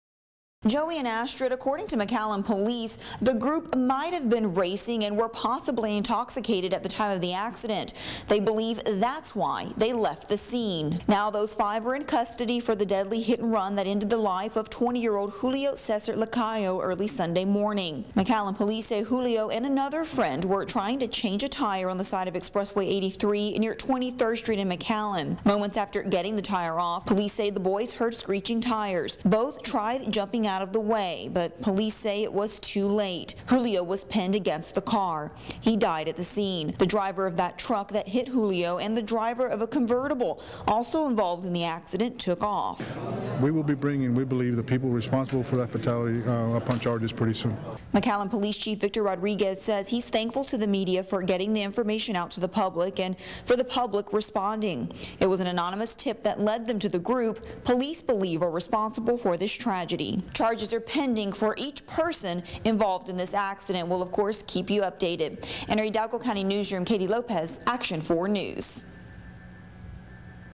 Five surrender for roles in McAllen fatal racing hit-and-run
Five people are in custody days after a father's plea for those who killed his son's friend on the side of U.S. Expressway 83 in McAllen.

The accident took the life of 20-year-old Julio Cesar Lacayo early Sunday morning.

Investigators told Action 4 News that tips allowed them to identify six people in four cars involved in the fatal accident.

As of Wednesday afternoon, four adults and one juvenile all thought to have been involved in the fatal hit-and-run have turned themselves in.

McAllen police believe the group might've been racing and were possibly intoxicated at the time of the crash where they all fled the scene.

Investigators said Julio and another friend were trying to change a tire on the side of Expressway 83 near South 23rd Street

Moments after getting the tire off, police said the boys heard screeching tires.

Both of them tried jumping out of the way but police say it was too late for Julio who got pinned against the car.

The 20-year-old died at the scene.

The driver of that truck that hit Julio and the driver of a Convertible also involved in the accident took off.

McAllen Police Chief Victor Rodriguez spoke to Action 4 News about the arrests.

"We will be bringing the people responsible for that fatality up on charges very soon," Chief Rodriguez said.

Rodriguez said he's thankful to the media for getting the information out to the public and for the public responding.

It was an anonymous tip that lead them to the group.

Investigators tell Action 4 News that the group will most likely be arraigned on Thursday morning.